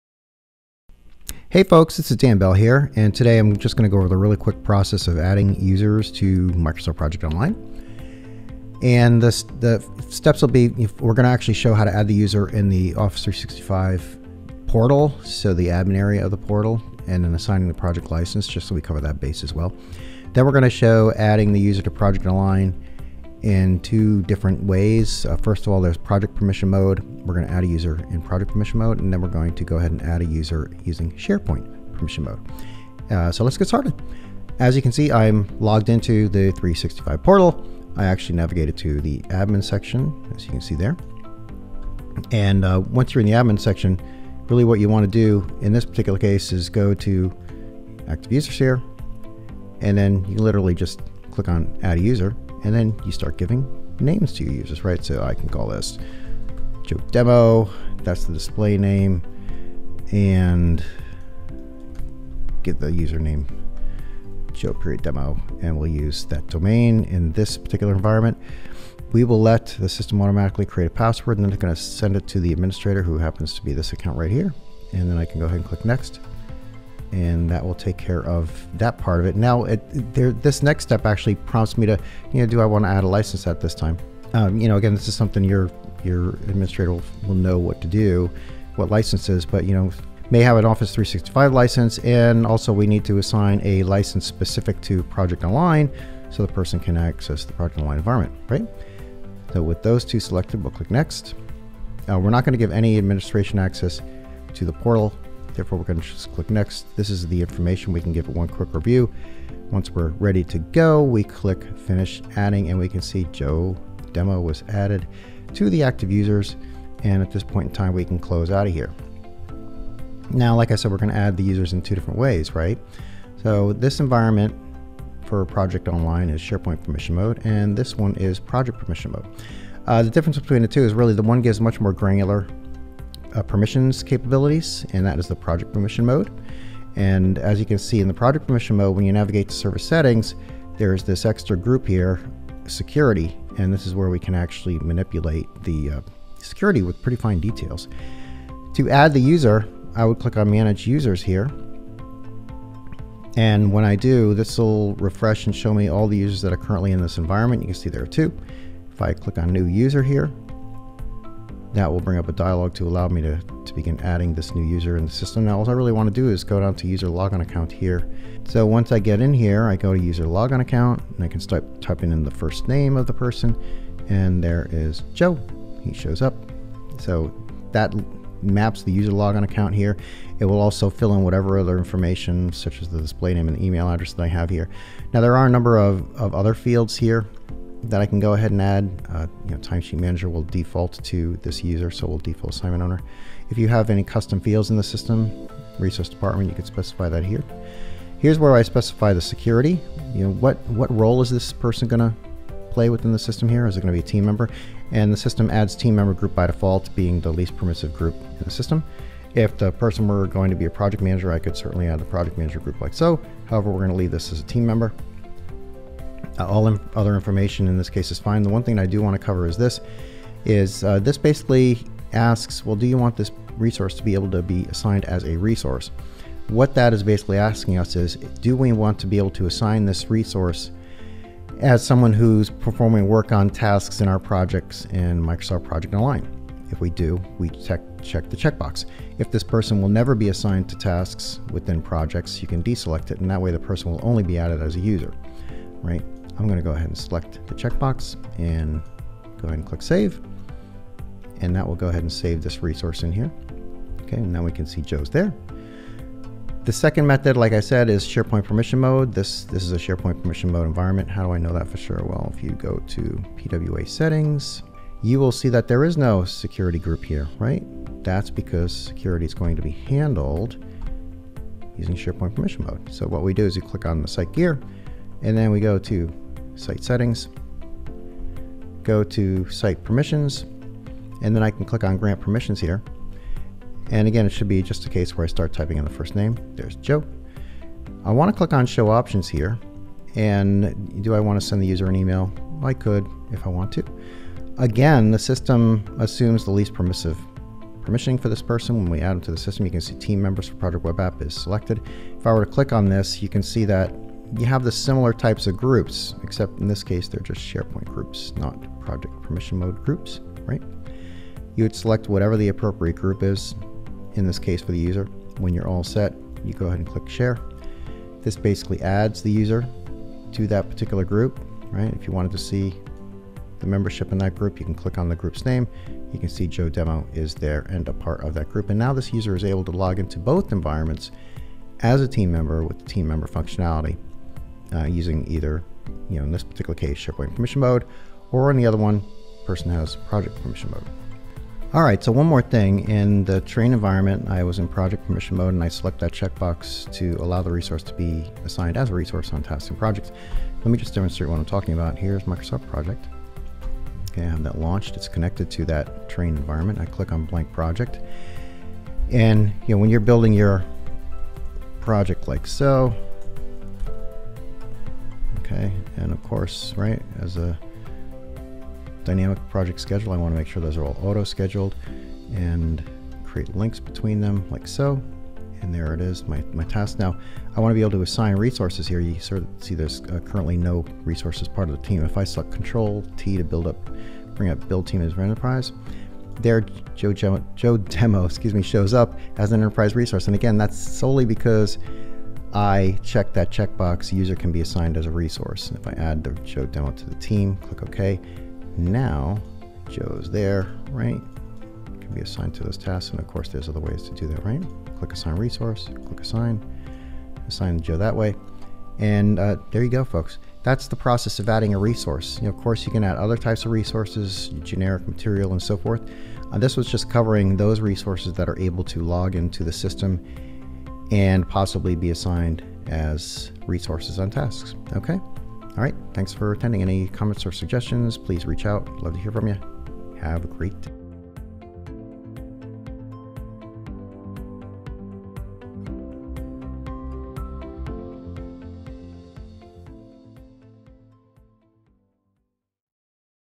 How to Add Users to Microsoft Project Online
ok, so you need to add users to the Enterprise Resource Pool in Microsoft Project Online?  The process is actually quite simple once you understand how to do it.  The process starts by adding users in the Microsoft 365 Admin center.  Be sure to assign the appropriate MS Project Online license when doing this part.

Once added to the portal, now your user may be added to Microsoft Project Online.  The process is different depending on whether you are in SharePoint Permission mode or Project Permission mode.  We cover both in this video.

SharePoint Permission Mode
In SharePoint Permission Mode select the gear in the top-right of the screen, then click the Site Settings link.  In the Users and Permissions section, select the Site permissions link.  From the ribbon select the Grand Permissions button.

In the Share SPM dialog box, begin typing the name of the users in the top text box.  As you type the user name those matching will show up in a list.  Select resource from the list.

Click on the Show Options link.  Consider the Send an email invitation checkbox.

Next, select the appropriate permission from the Select a permission level drop down list box.  typically Team Members, Project Manager or Administrator will be used most often.  Click the Share button when completed.

Project Permission Mode
In Project Permission Mode there is a Security group of command in PWA Settings.  This particular permission mode allows for very granular permissions for your users.   Think visibility based on reporting structure using a Resource Breakdown Structure (RBS).  This is my preferred mode simply because it's very powerful, and, why not?

To add a user in this mode select PWA Settings.  Then in the Security grouping of commands click Manager Users.  Next you will want to click the New User button.  The first item you want to focus on in the New User screen is the User logon account (User account).

What should happen is users who's name match what you are typing will show up.  Select the correct user from the list of users.

The next section that is important is the Security Groups section.  By default the system will have just the Team Members added to this user in the Groups that contain this user text box.  If this person is to be a Team Member you would leave as-is.

However, if the user is to be a Project Manager, simply select the Project Manager groups from the Available Groups dialog box, then click the greater than sign button.  This will add the group to the Groups that contain this user section.  You may want to remove the Team Members group while you are here.

In the identification section note the checkbox for User can be assigned as a resource.  This is specifying whether the user can be added to a project using the Build Team from Enterprise dialog as well as assigned to tasks in project.  With the checkbox unchecked, the user will be able to login to the system, however, the user will not be able to be assigned to project tasks.  Make the appropriate selection based on the requirements for the user.

All other settings on the New User screen are dependent on your organizations user and configuration of the system.  If there are Resource Fields or Custom Fields to be assigned, you may do that here.  Otherwise, click the Save button to save all the specified settings for the user.

In Project Permission Mode you may also use the Resource Center to add users.  From the Resource Center click on the New button in the Resources section of the ribbon.  Other than not having the ability to select what security group the user belongs to you now have a very similar screen to what you had in Manage Users.  Use the same procedure outlined above and Save when finished.

Use the Manage User or Manager Groups links in the Security grouping to manipulate the resource security level.

Microsoft Project Professional

the Microsoft Project client may be used to bulk edit resources using the Resource Sheet instead of Project Web App.  You merely select all the resources you want to edit from the Resource Center, then click the Open in MS Project Professional button.